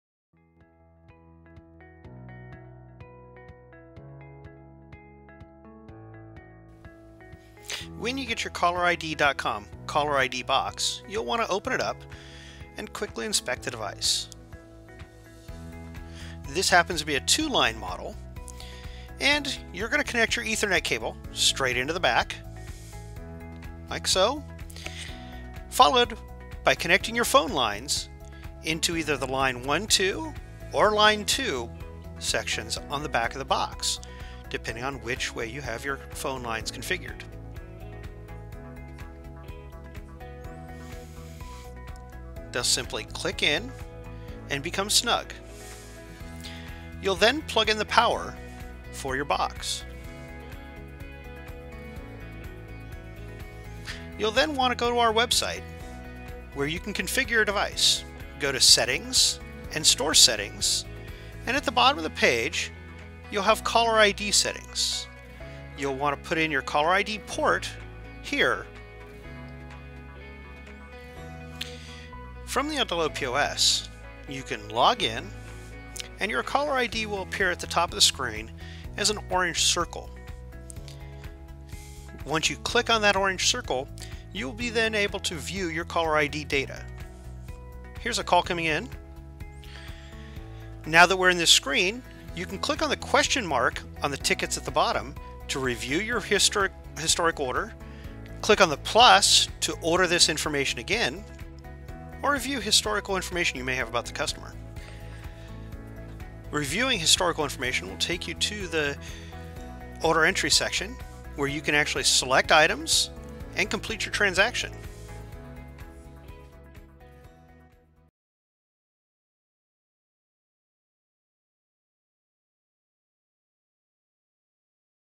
Caller Id
Aldelo Express Point of Sale with Caller Id.com 2 line caller id box is perfect for take out of delivery options and this quick videos shows you how to setup the caller id and basic use in the point of sale